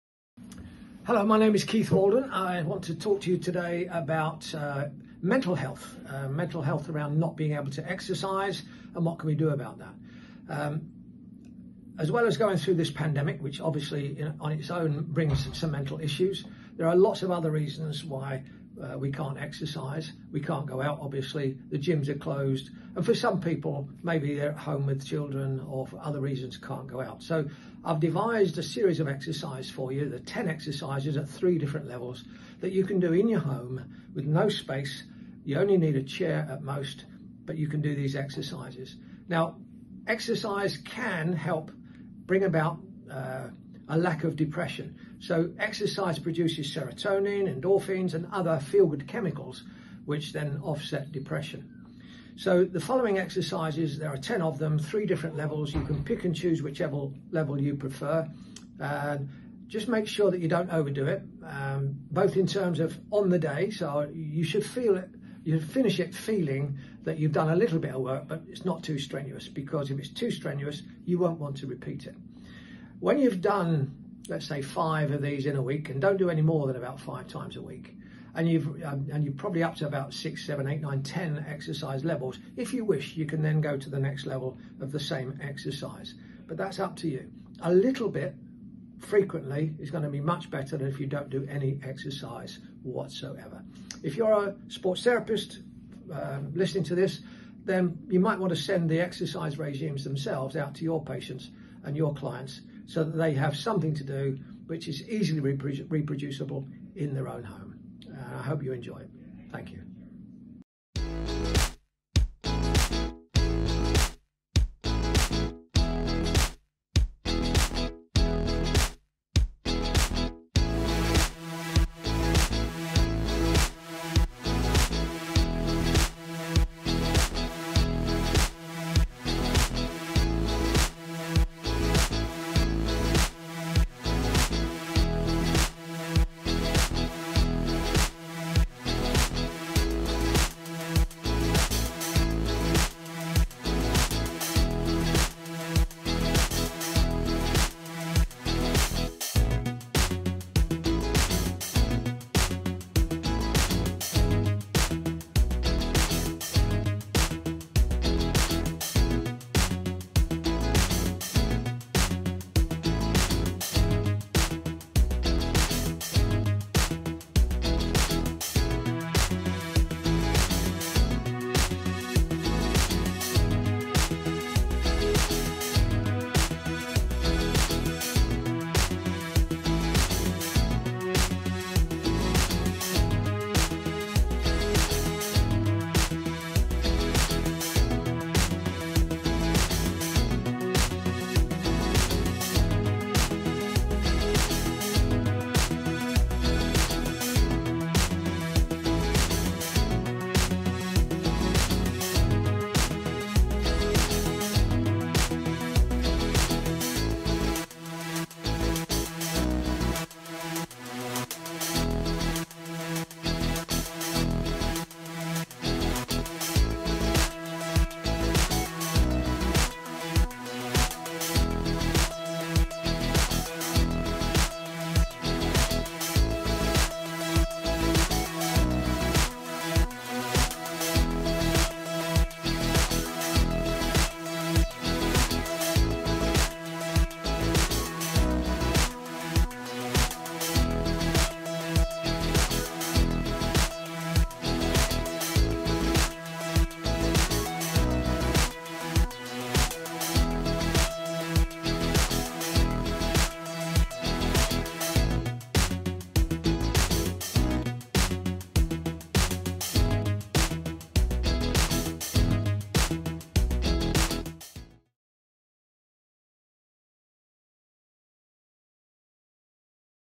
At Home Exercises | Time to Talk Day
Keith Waldon together with Graduate Members George, Emily and Steve, all therapists from his clinic, demonstrate some exercises to keep active during these difficult times. James gets behind the camera.